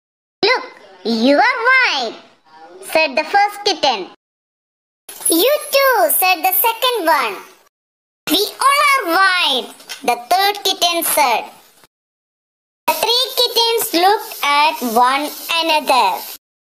THREEKITTENS
പൂച്ച കുട്ടികൾ എന്താണ് പറഞ്ഞത് ? കേട്ടു നോക്കാം  II STD 1 UNIT 4  THREE KITTENS II KITE VICTORS